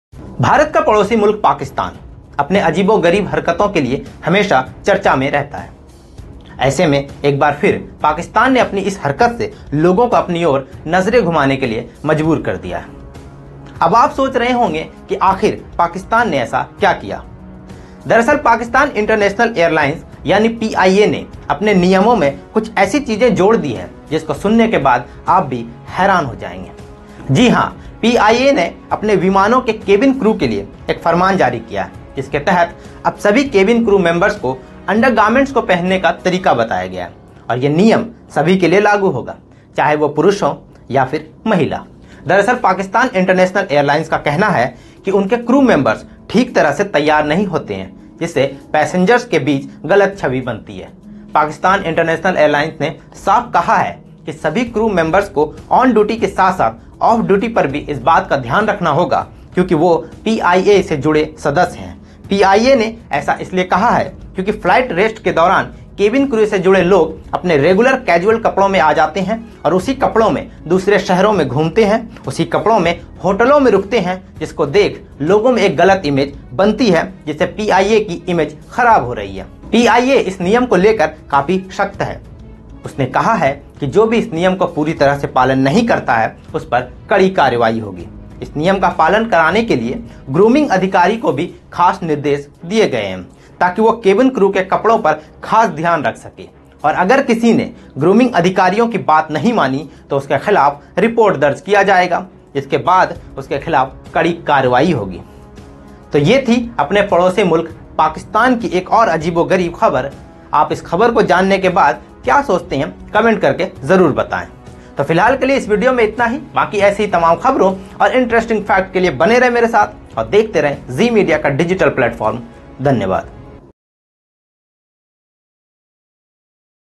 Pakistan Air Hostess New Rules | पाकिस्तान International Airlines का अजीबो गरीब फरमान! | PIA
भारत का पड़ोसी मुल्क पाकिस्तान अपने अजीबो-गरीब हरकतों के लिए हमेशा चर्चा का विषय बना हुआ रहता है.ऐसे में एक बार फिर पाकिस्तान ने अपनी इस हरकत से लोगों को अपने और नजरें धुमाने के लिए मजबूर कर दिया है. अब आप सोच रहे होंगे कि आखिर पाकिस्तान ने ऐसा क्या किया है, दरअसल पाकिस्तान इंटरनेशनल एयरलाइंस यानी PIA ने अपने नियमों कुछ ऐसी चीजें जोड़ दी है जिसको सुनने के बाद आप भी हैरान हो जाएंगे, जी हां PIA ने अपने विमानों के केबिन क्रू के लिए एक फरमान जारी किया है जिससे तहत अब सभी केबिन क्रू मेंबर्स को अंडरगार्मेंट्स को पहनने का तरीका बताया गया है